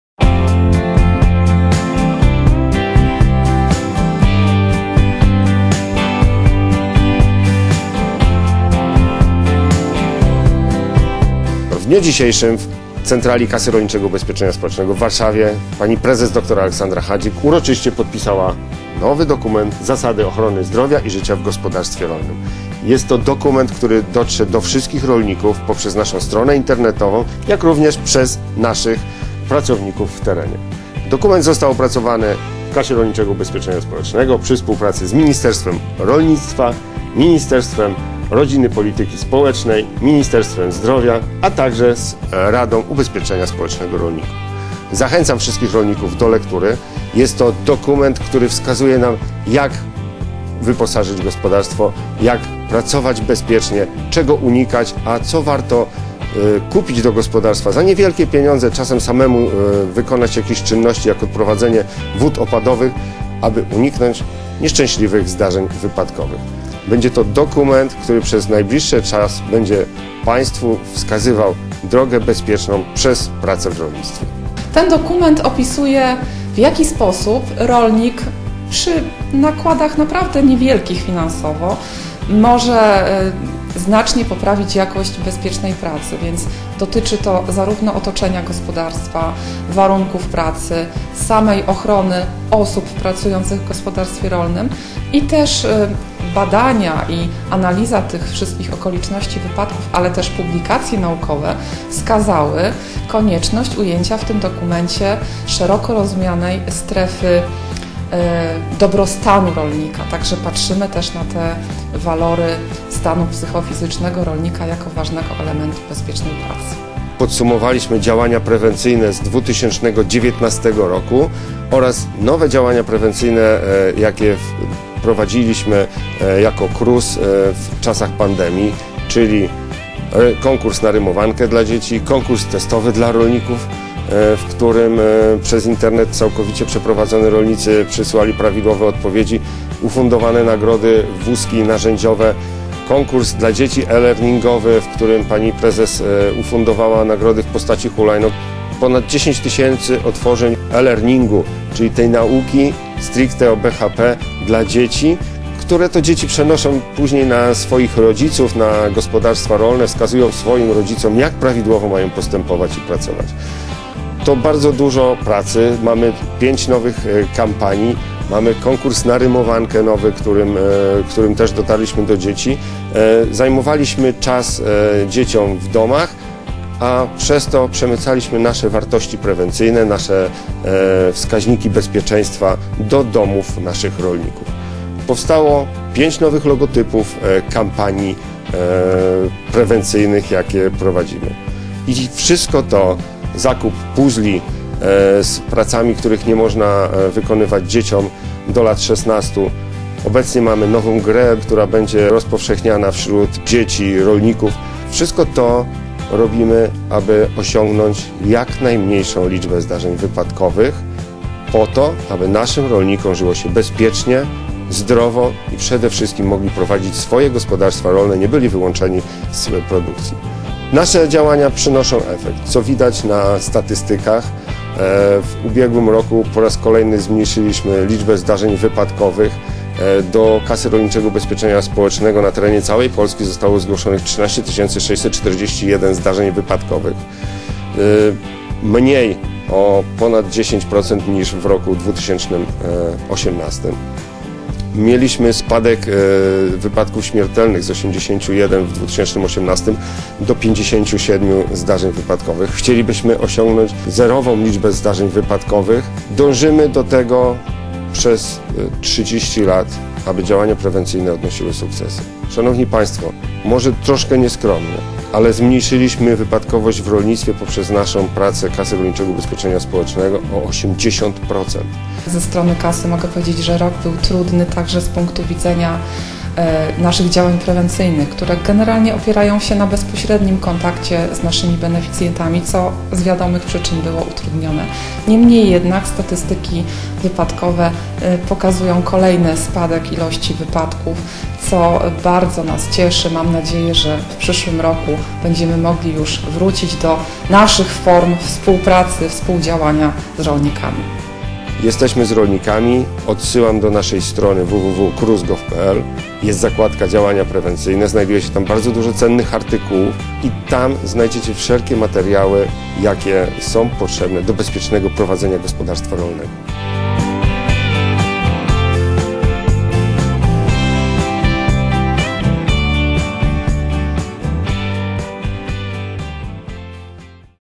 Działania prewencyjne Kasy Rolniczego Ubezpieczenia Społecznego w 2020 roku
10 grudnia br. w Centrali Kasy Rolniczego Ubezpieczenia Społecznego odbyła się konferencja Prezes KRUS dr Aleksandry Hadzik poświęcona działaniom prewencyjnym KRUS w 2020 roku.
Podczas wydarzenia Prezes Kasy dr Aleksandra Hadzik ogłosiła również zaktualizowany dokument Zasady ochrony zdrowia i życia w gospodarstwie rolnym.